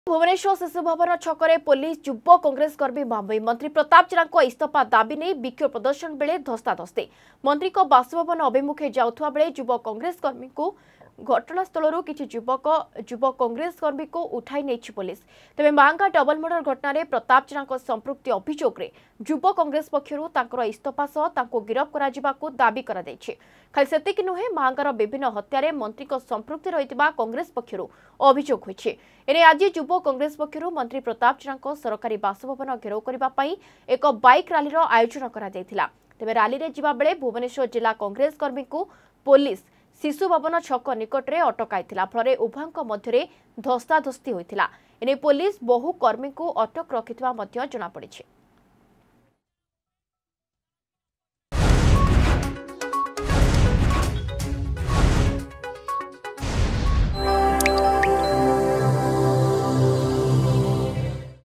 ଭୁବନେଶ୍ବର ଶିଶୁଭବନ ଛକରେ ପୋଲିସ-ଯୁବ କଂଗ୍ରେସ କର୍ମୀ ମୁହାଁମୁହିଁ
ମନ୍ତ୍ରୀ ପ୍ରତାପ ଜେନାଙ୍କ ଇସ୍ତଫା ଦାବି ନେଇ ବିକ୍ଷୋଭ ପ୍ରଦର୍ଶନ ବେଳେ ଧସ୍ତାଧସ୍ତି । ମନ୍ତ୍ରୀଙ୍କ ବାସଭବନ ଅଭିମୁଖେ ଯାଉଥିବା ବେଳେ ଯୁବ କଂଗ୍ରେସ କର୍ମୀଙ୍କୁ ଘଟଣାସ୍ଥଳରୁ କିଛି ଯୁବ କଂଗ୍ରେସ କର୍ମୀଙ୍କୁ ଉଠାଇନେଲା ପୋଲିସ.